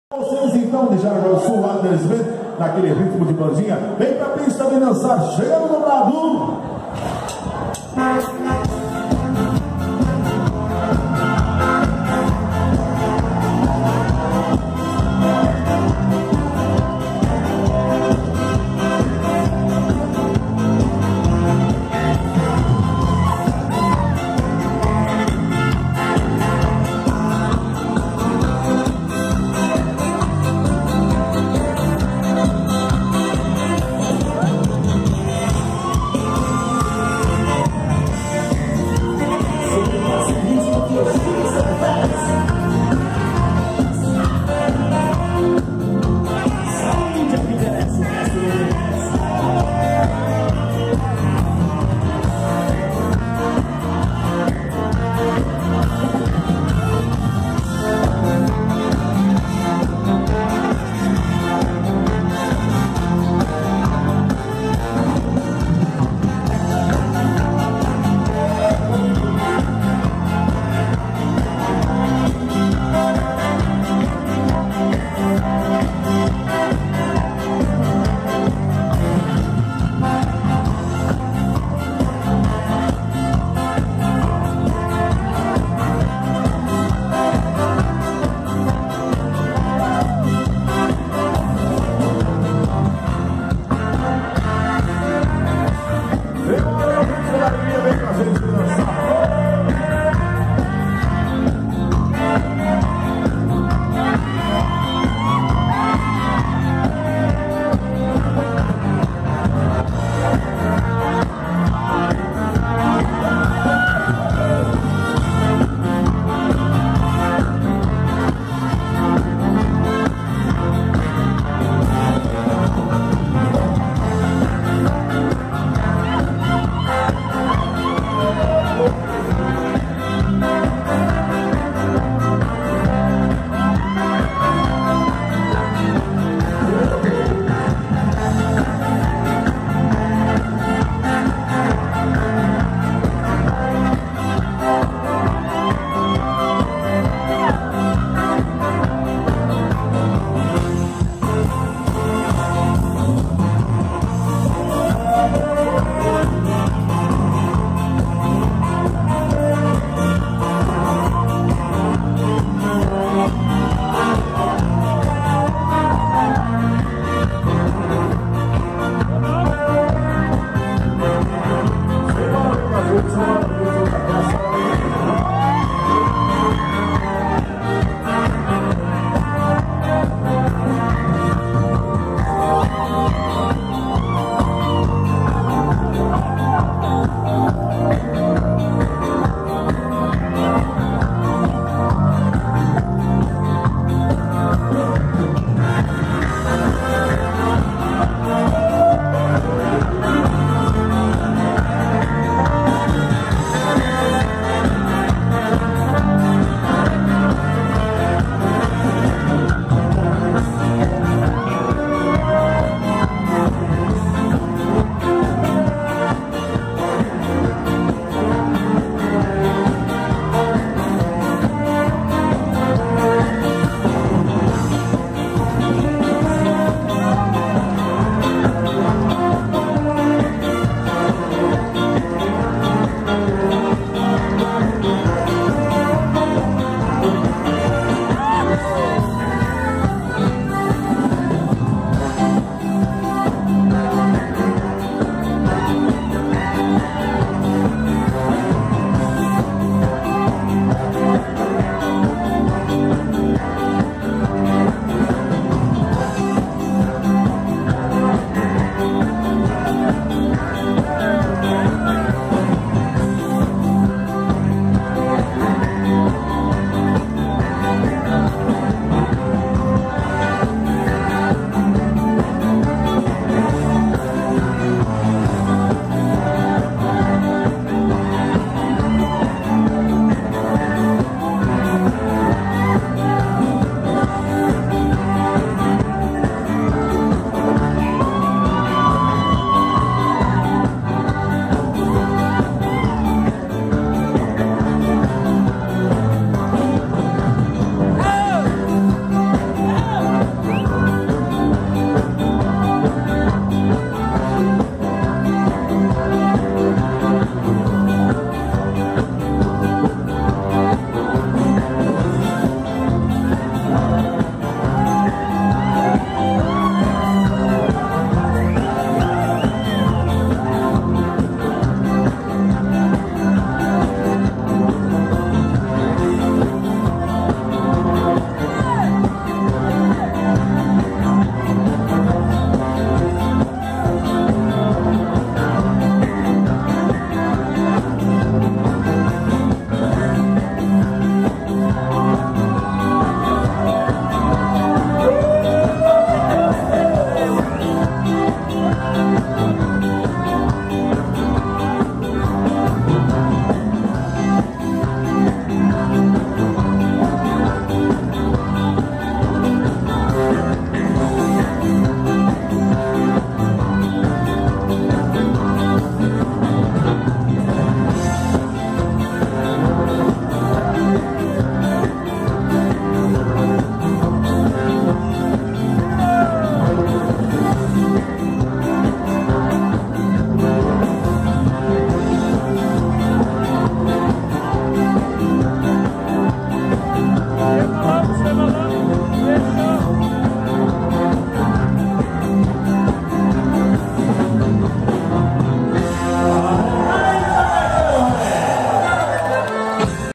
Adlers Band -Sociedade 25 de Julho – Jaraguá 99 – Schützenverein
Festa deRei de Rosane e Ireneu Wulff na Sociedade 25 de Julho – Jaragu á 99 – Schützenverein